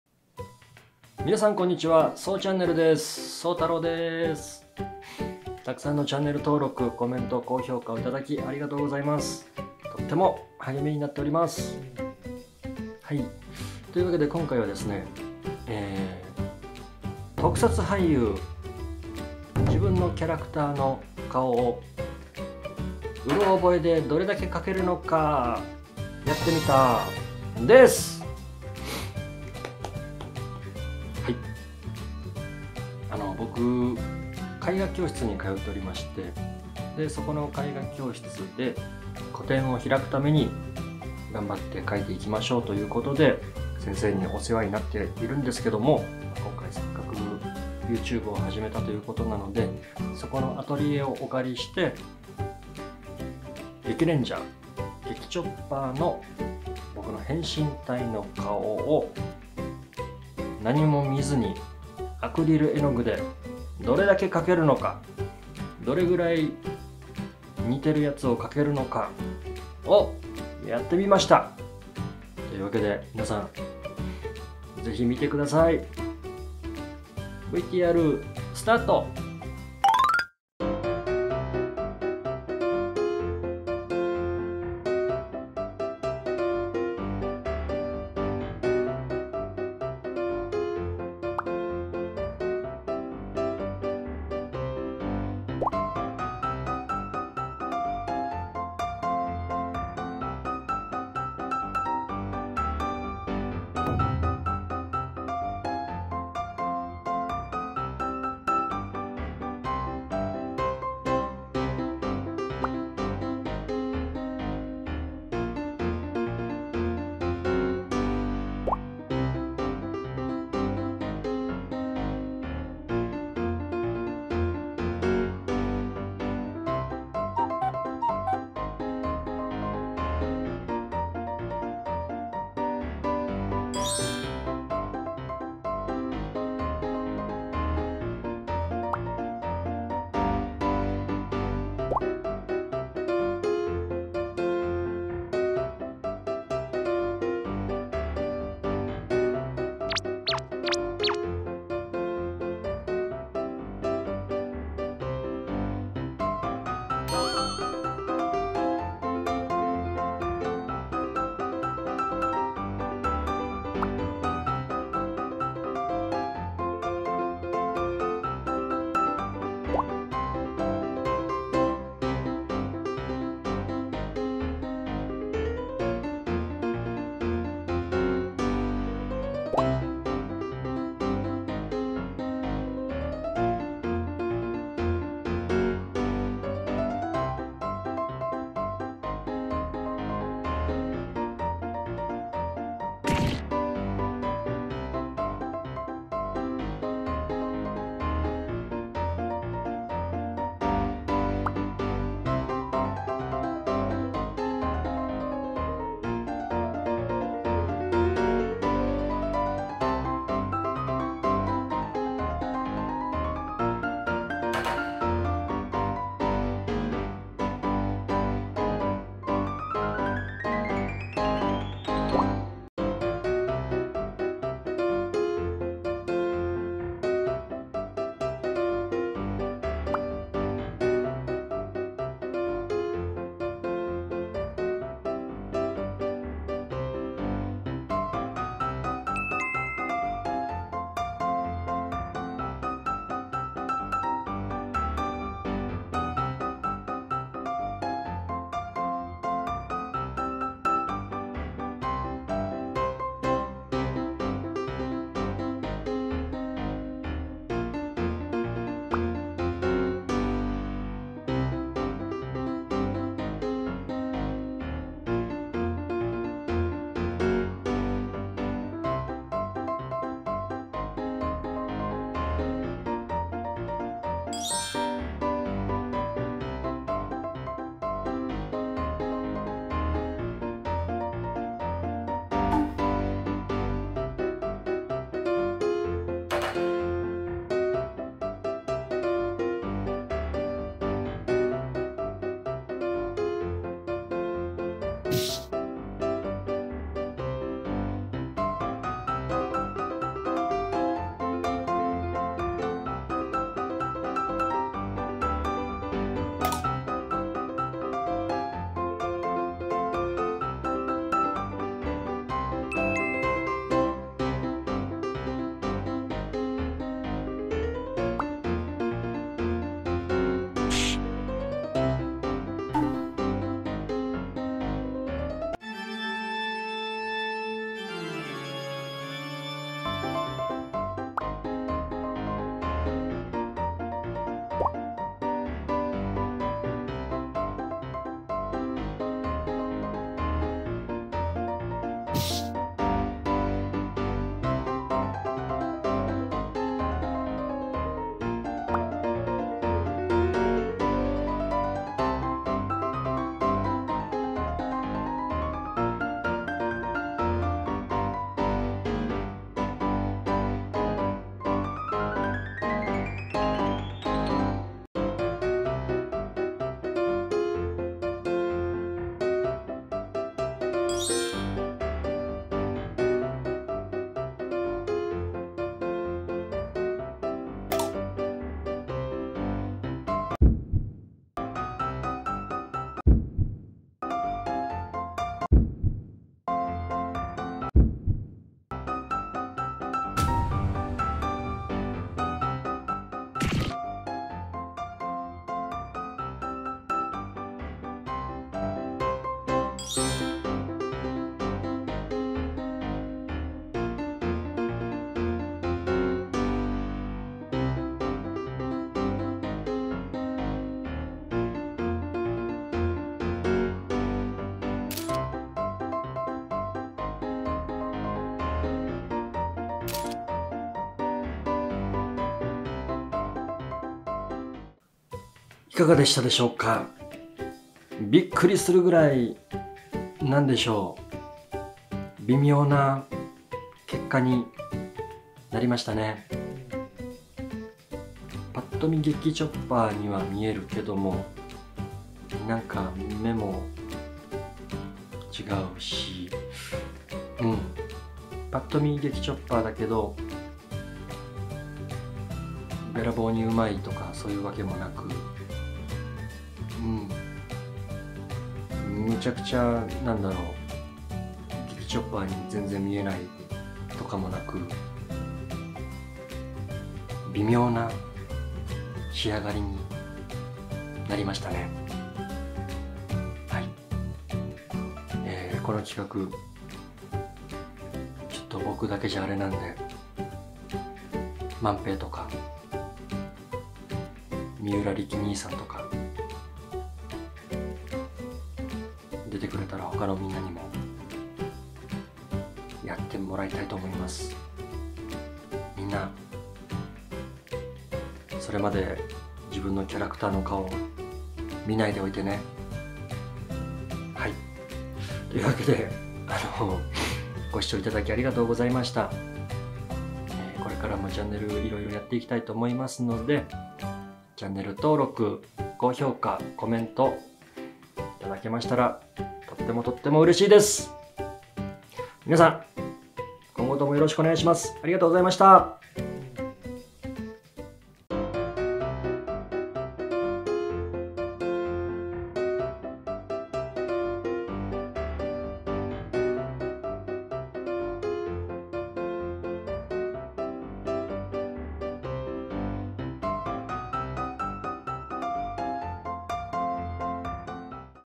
【ゲキチョッパー】自分の変身体をカンニングなしで描いてみた
聡太郎です！
今回は、自分の変身したキャラクター・ゲキチョッパーの顔を
お世話になっているアトリエCHICORAさんのアトリエをお借りして
何も写真資料を見ずにどれだけ描けるのか、チャレンジしてみました！
最後までお楽しみいただければ幸いです！